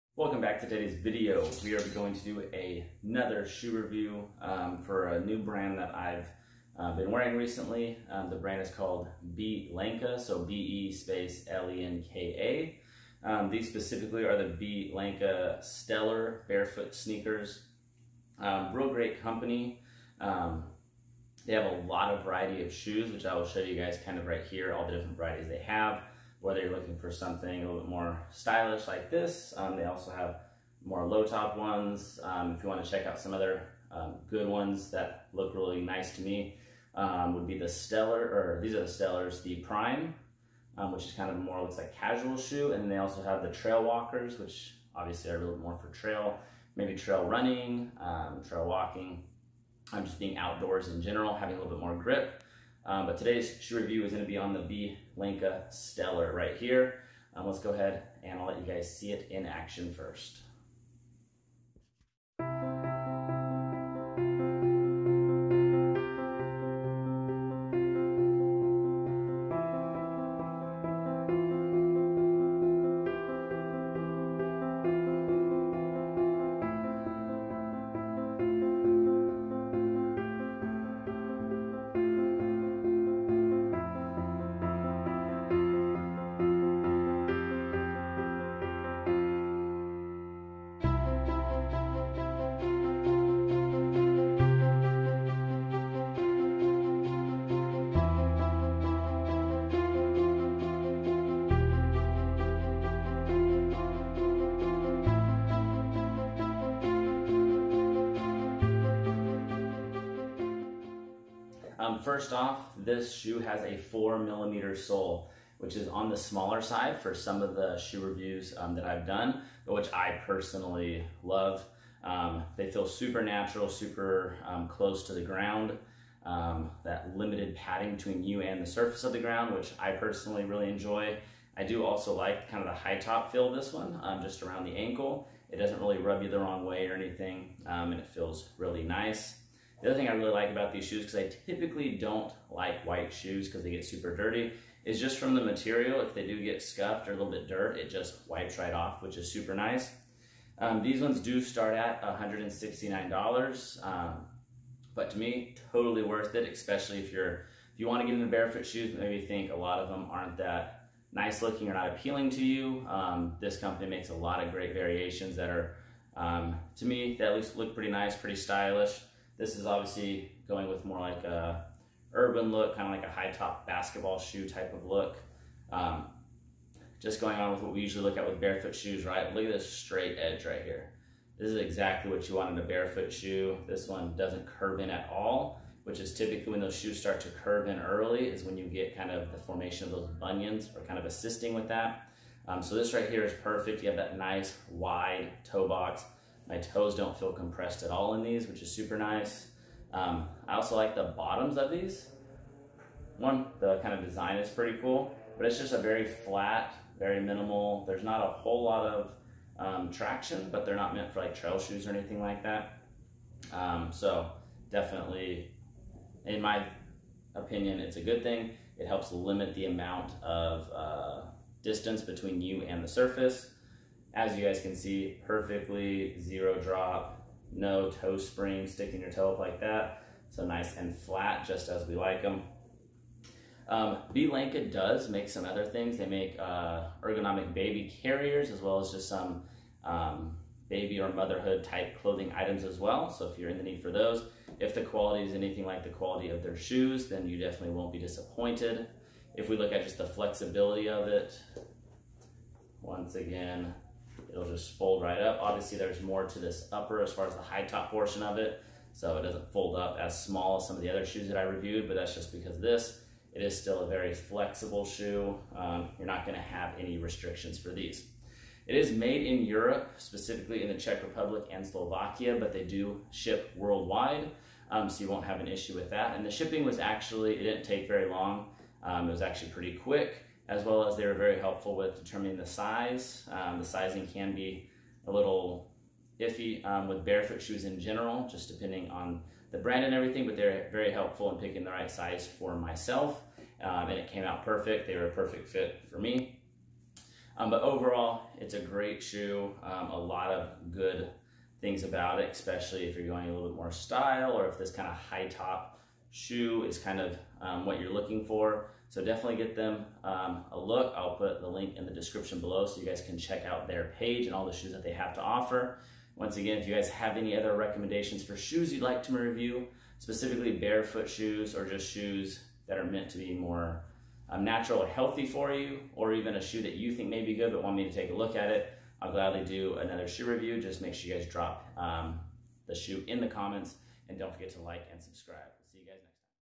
Be Lenka Barefoot Sneaker Review!!!!
The Be Lenka Stellar Sneaker is one of the coolest barefoot shoes that I have. They are super comfortable and a well made shoe that hits all the key features of a good barefoot shoe including a 4mm sole!!! Hope you enjoy this shoe review and make sure to get yourself a pair if this is your style.